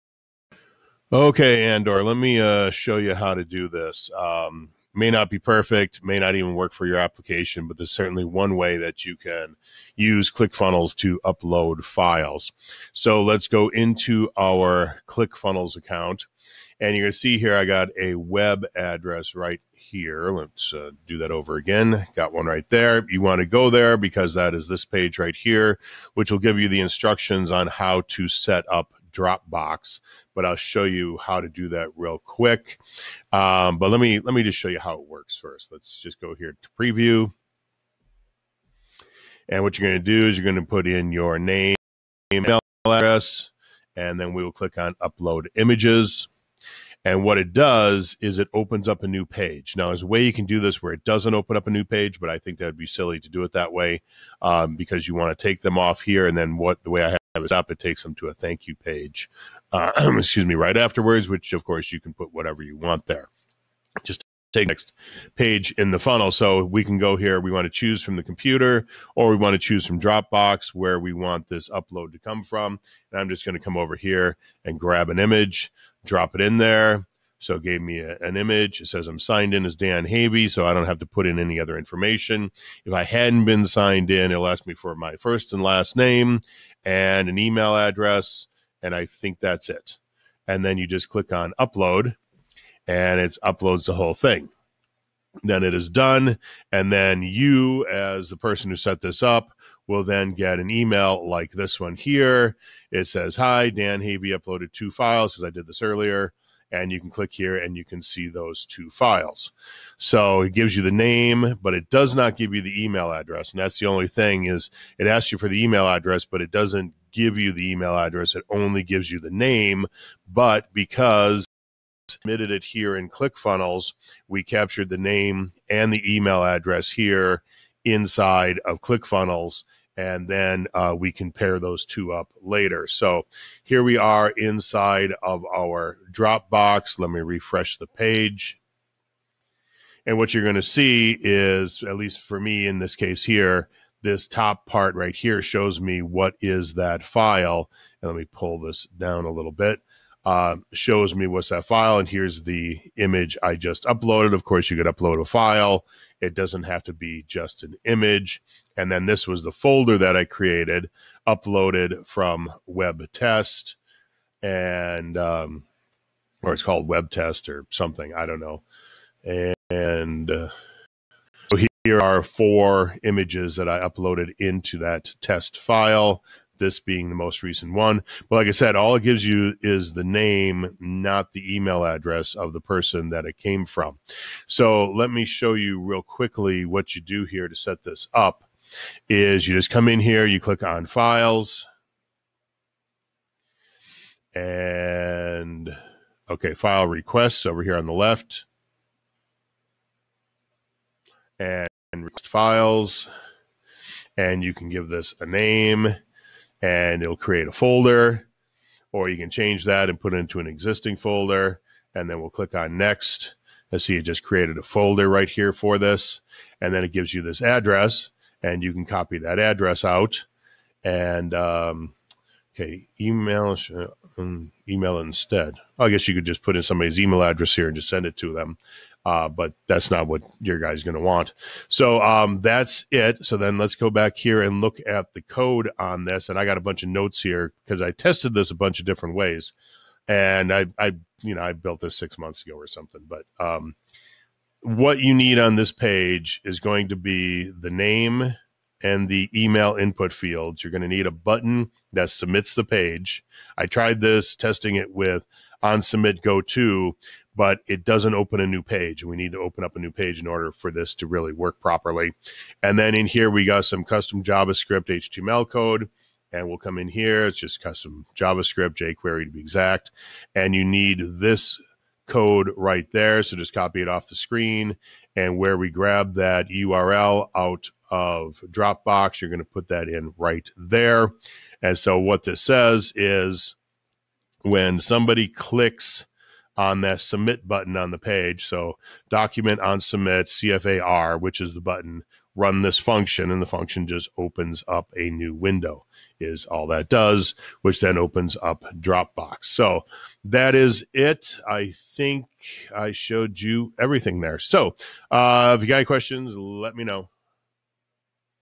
How To Have Your Consumer Upload Images To Dropbox From Your ClickFunnels Page - CFDS-27
I often get the request to be able to have people upload images or even documents from a ClickFunnels Page.

This is not a Native Function of ClickFunnels but can easily be done in combination with Dropbox.

Follow along in the video as I show you haw to set this up in ClickFunnels and Dropbox.